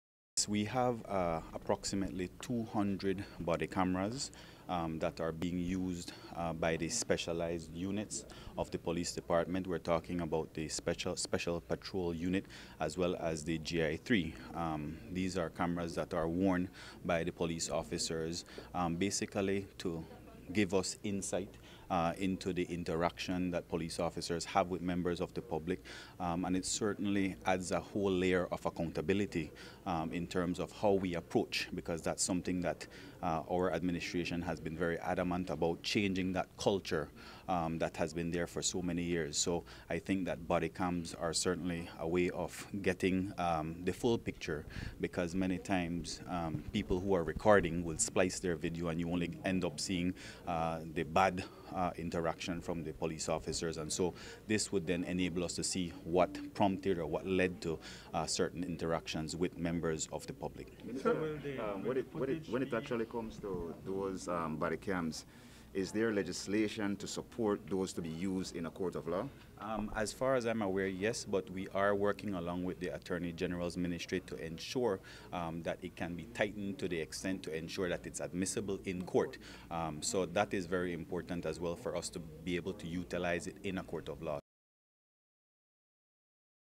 Special Police Units Using Body Cams
While the Belize Police Department received a virtual forensic laboratory to assist in combating cybercrimes, as we found out today from Kareem Musa, specialised units within the department have been using body cameras for some time now while performing their duties. The Home Affairs Minister says that it provides a different perspective when there are confrontations with the officers and that they are working with the Attorney General’s Ministry to ensure that video footage can hold water during court proceedings.